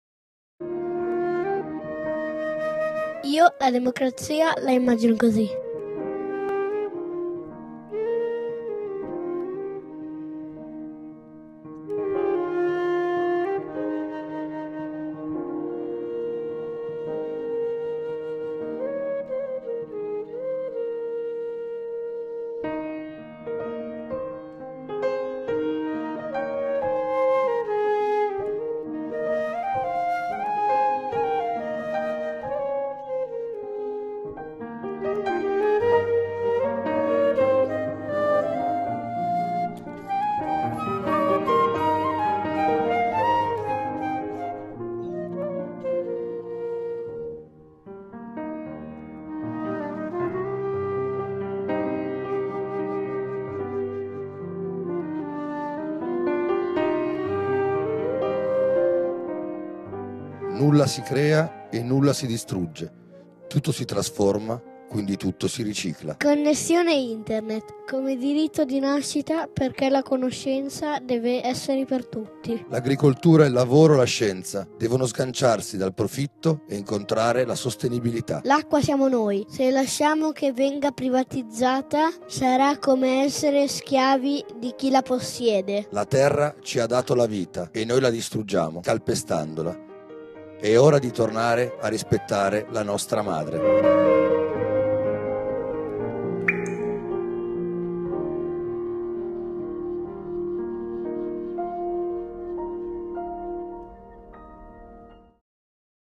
La Democrazia, io la immagino così...
Il Movimento 5 Stelle nasce dai cittadini che hanno deciso di riappropriarsi delle istituzioni. L'obiettivo principale del nostro MoVimento è ristabilire un controllo etico da parte dei cittadini nei confronti della politica e dei poteri forti. Quando la costituzione sarà veramente applicata, anche grazie al lavoro svolto nel MoVimento dalle persone che donano il proprio tempo per cambiare questo sistema malato, tutti noi potremo
finalmente tornare a vivere le nostre vite come cittadini e non come sudditi.